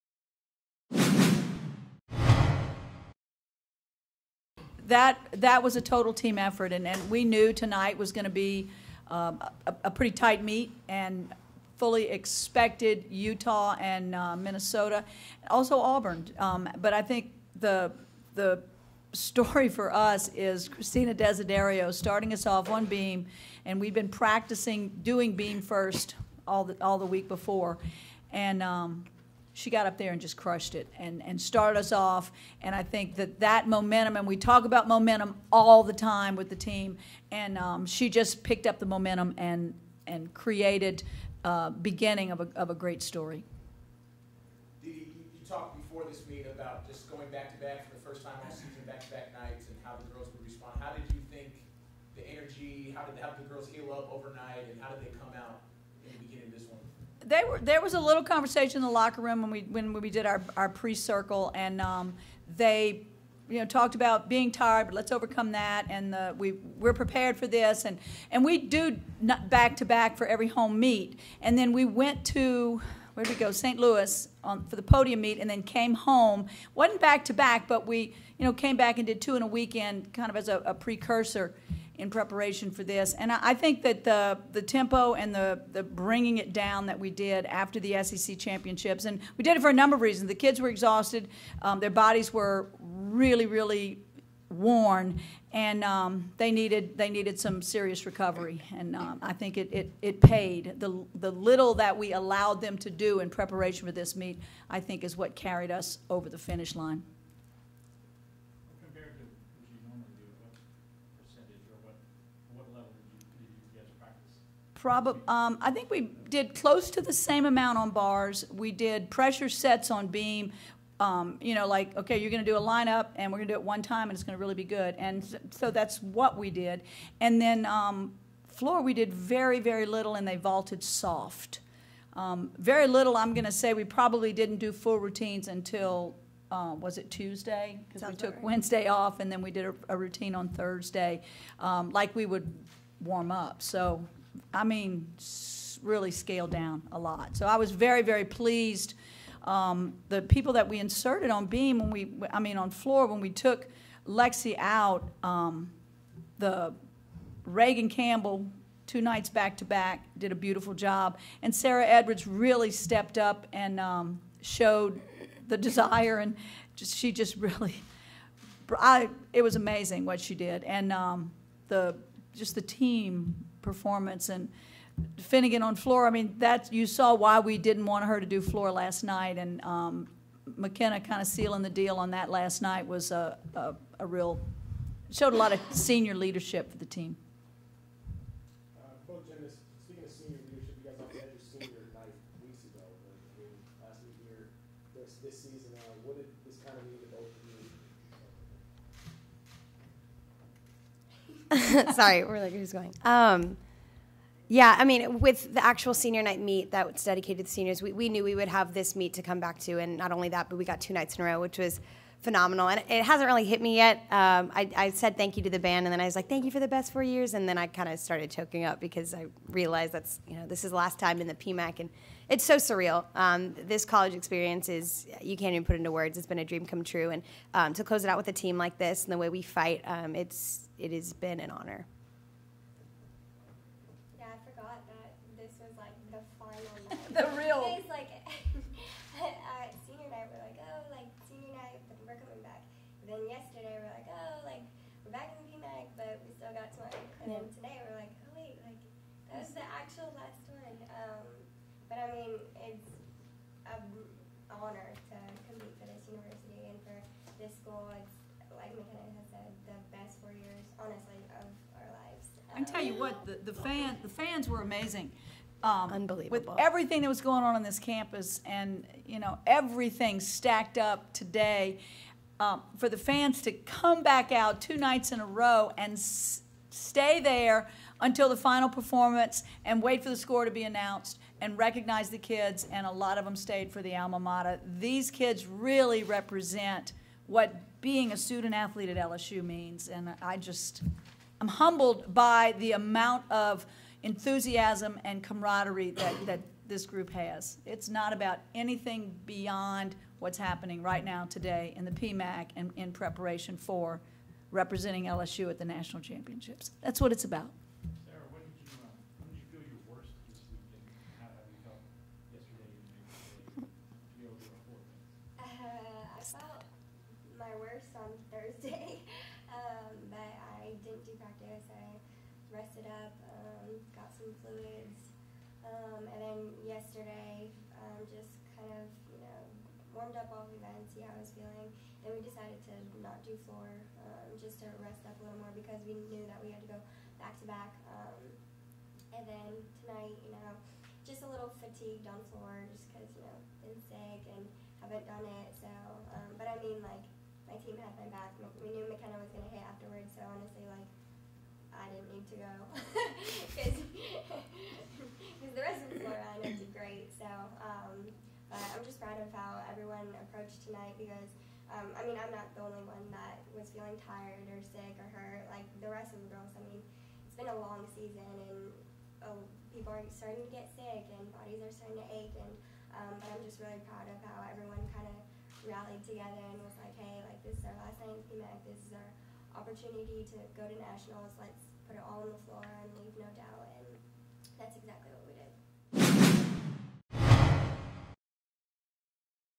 FULL SOUND: "That was a total team effort" LSU Gymnastics advances to finals
HC D-D Breaux discusses LSU's win and advancement to the NCAA Finals along with seniors Sarah Finnegan and McKenna Kelley